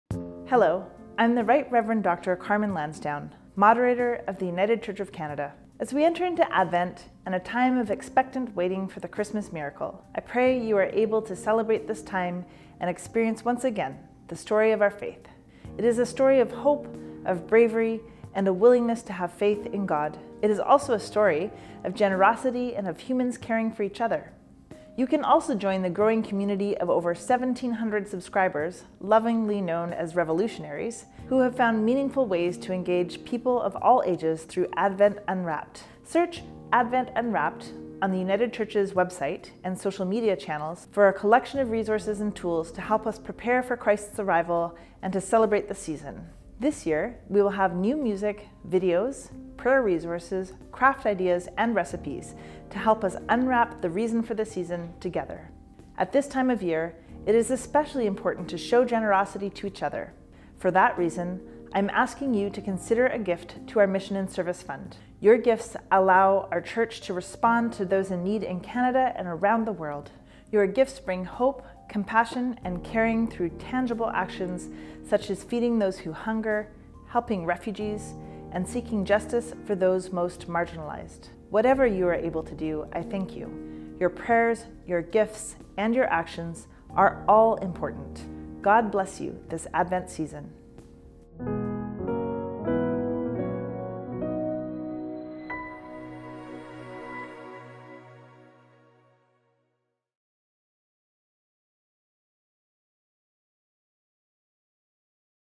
Advent Message 2023 - Moderator Carmen Lansdowne
The Rt. Rev. Carmen Lansdowne, Moderator of The United Church of Canada, shares her Advent message for 2023. Advent is a time of expectant waiting for the Christmas miracle - a story of hope, bravery, and the willingness to have faith in God. It is also a story of generosity and of humans caring for each other.